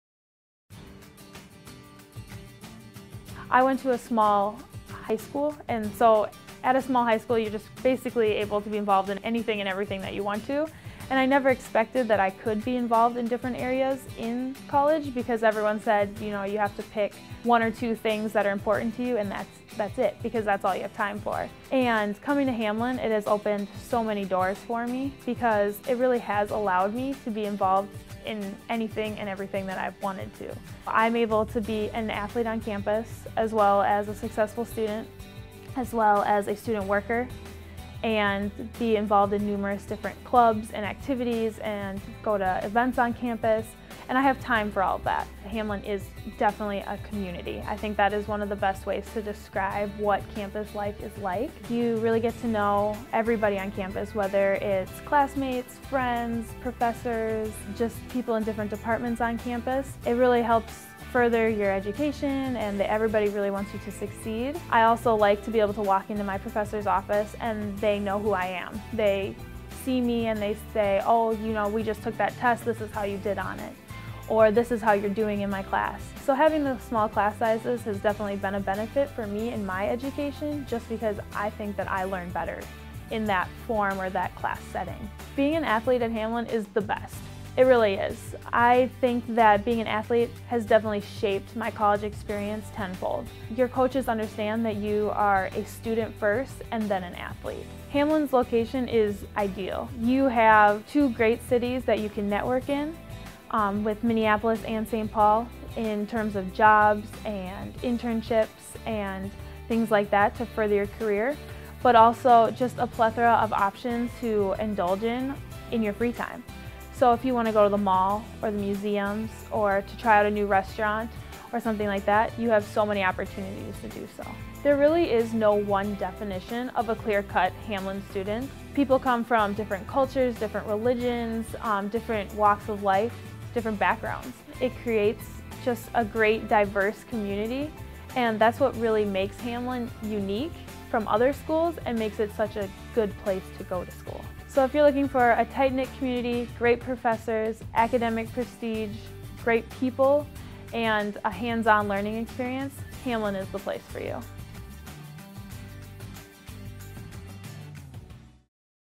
Make Your Mark: Rachel Usgaard
Biochemistry and psychology student Rachel Usgaard talks about what life as a Hamline student is really like, as a student, an athlete, and an active member of organizations and events both on and off-campus. More